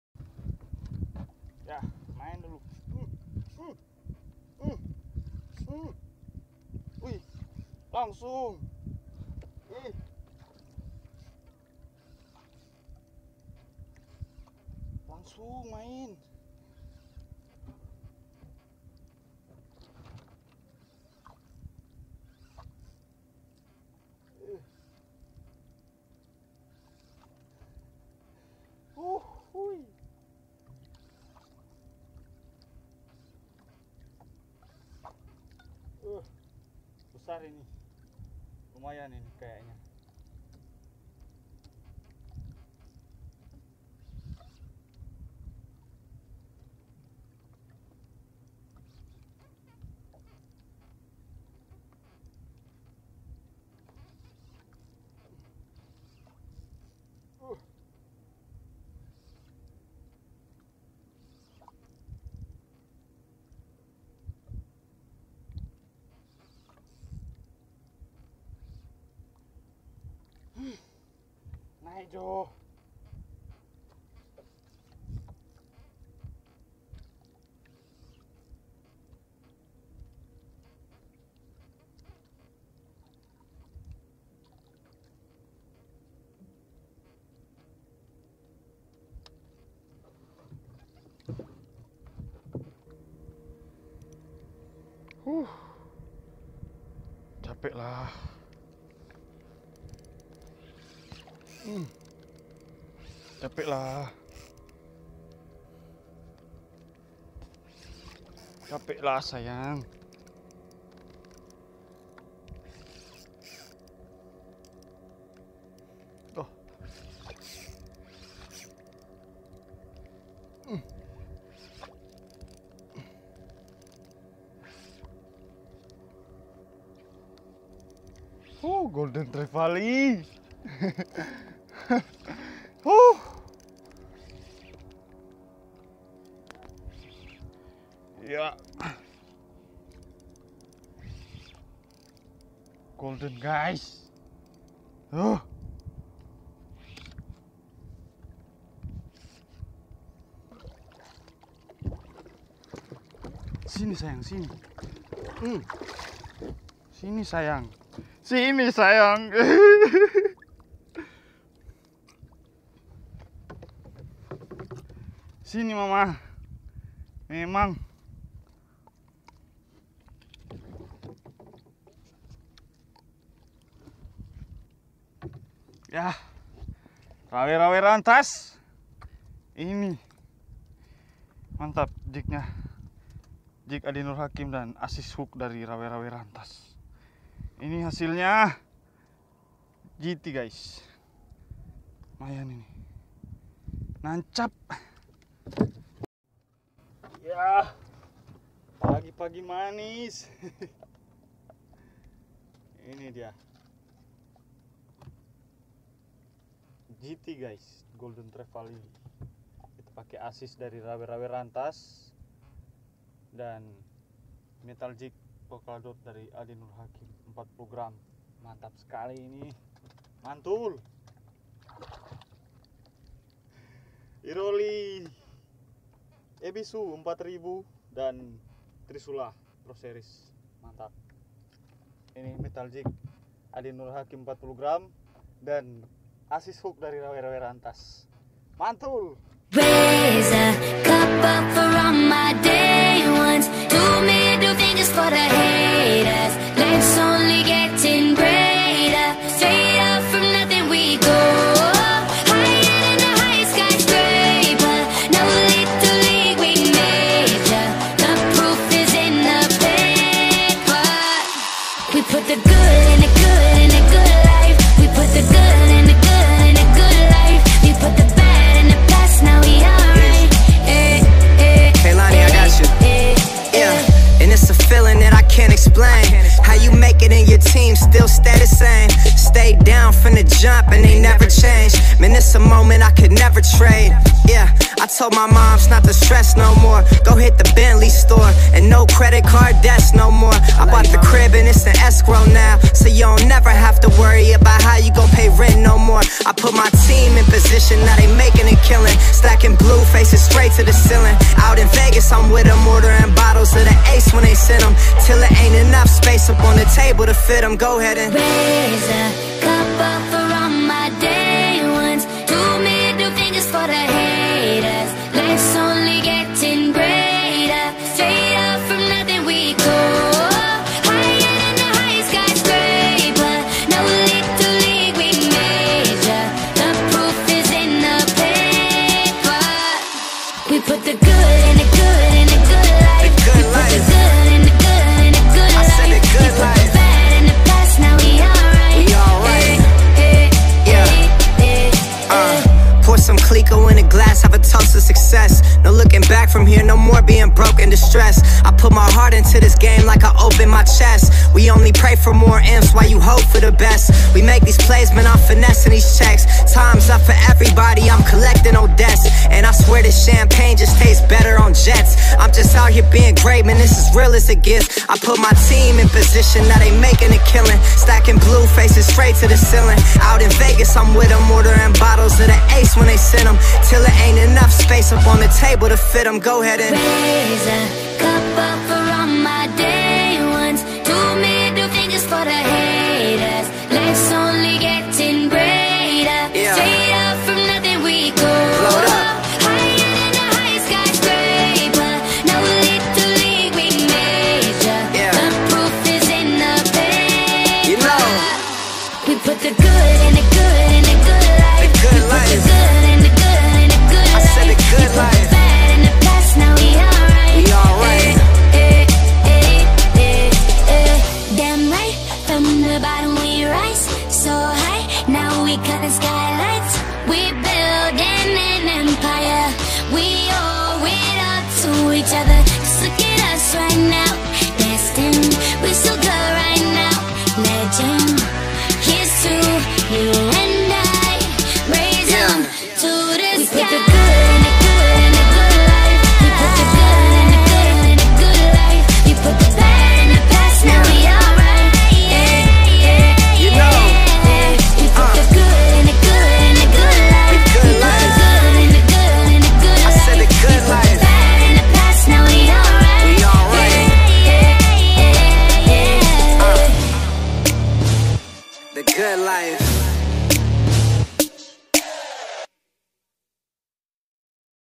Jig Langsung di sambar Mama GT, Auto Betot 🐋
Salam Jumpa Lagi dati Pejuang DaratLautUdara 😁
Mancing Lightjigging di Perairan Minanga, Pusomaen.  Minahasa Tenggara

Bantu Support 🙏🏻
Supaya Makin Berkembang 🤗💪

Set :
Rod Daido Trisula
Reel Iroly Ebisu 4000
Line Falcon
Leader Shock RN
jig 40gr
Assisthook Rawerawe Rantas 1/0